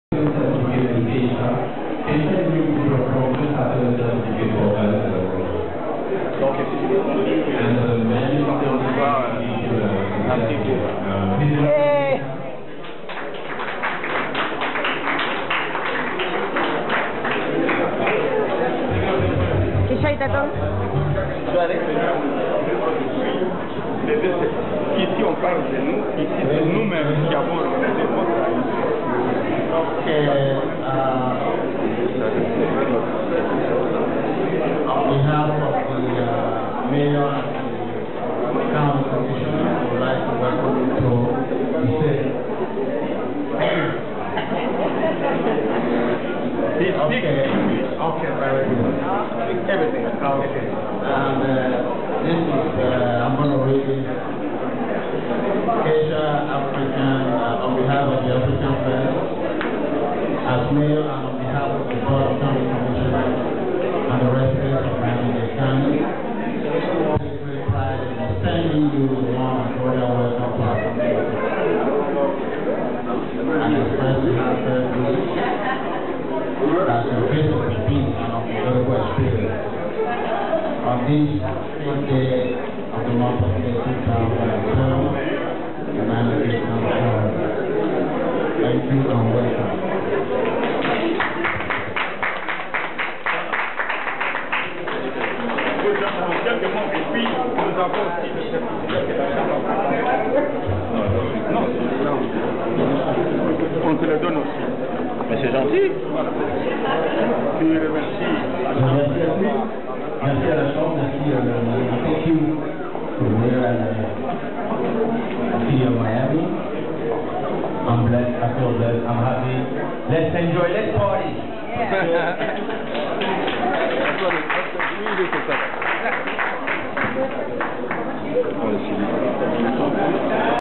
Kaysha recieves distinguished visitor in Miami (05/07)
Kaysha recieves distinguished visitor by the city of Miami at Marlyn hotel. (05/07)